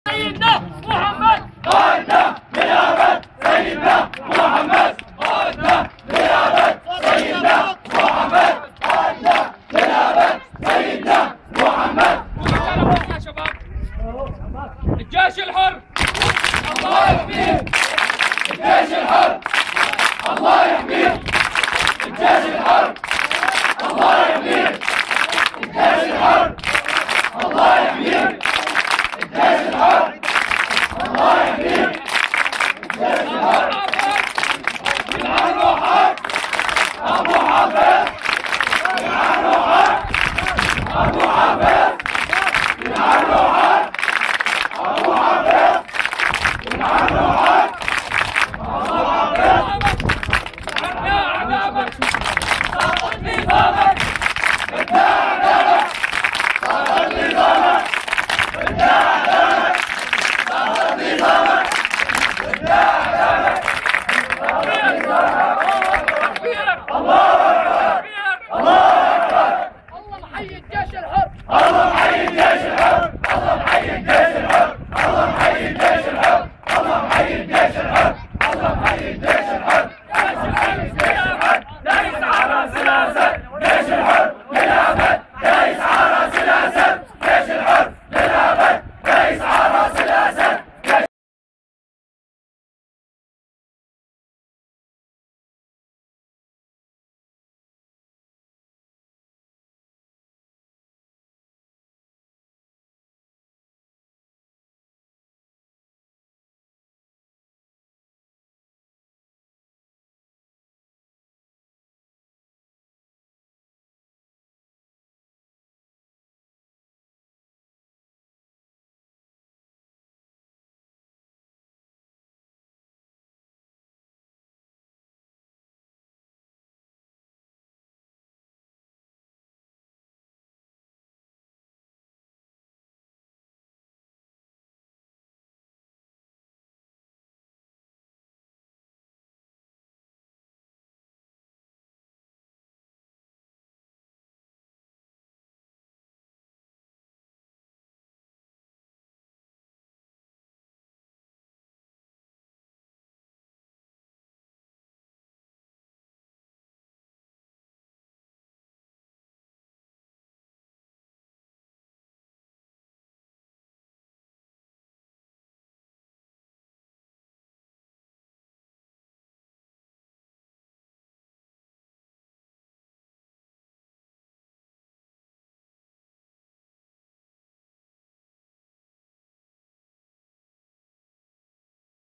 حلب :: شدار 8-6-2012
مظاهرة تخرج لأول مرة في هذه البلدة وهذا المقطع وصل متاخر للاسف ولذلك نرجوا الاهتمام به